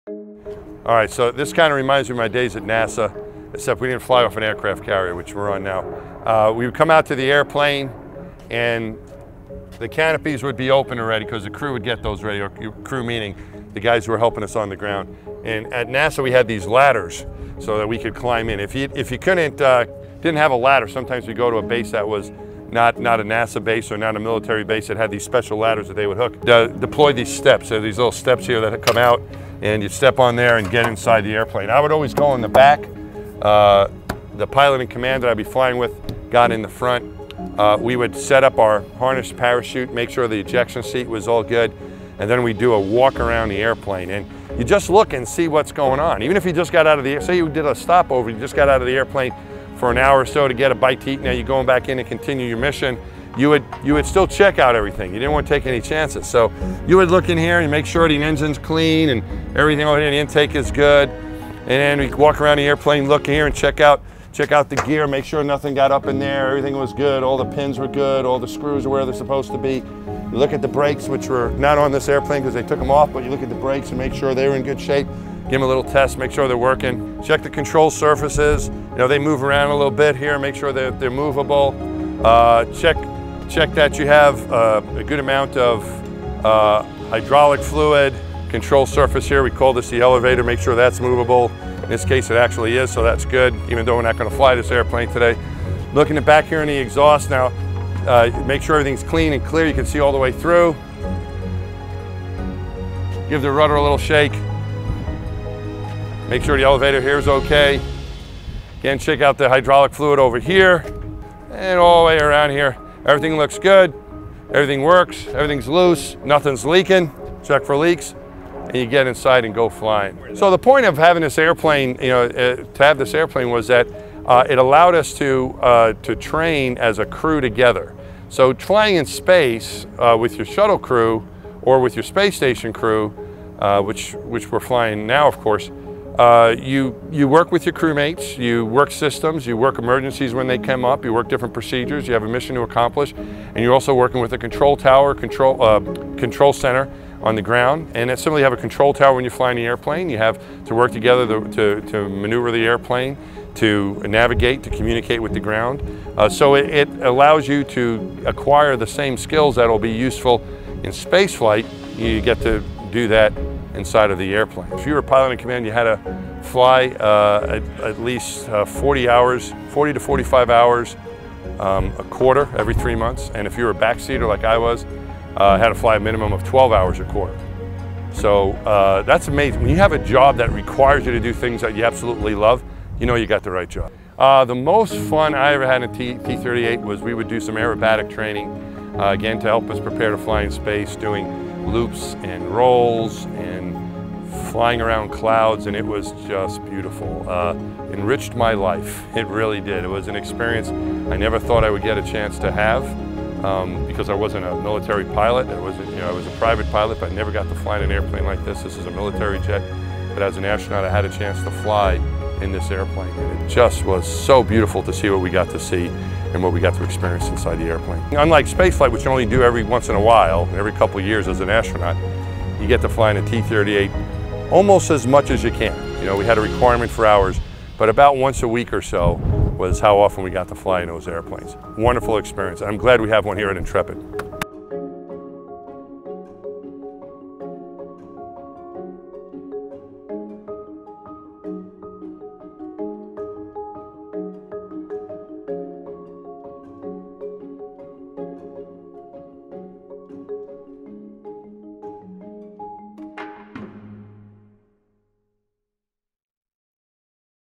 Behind the Scenes: T-38 Talon with Mike Massimino
Our Senior Space Advisor Mike Massimino takes us behind the scenes of the T-38 Talon we have on our Flight Deck.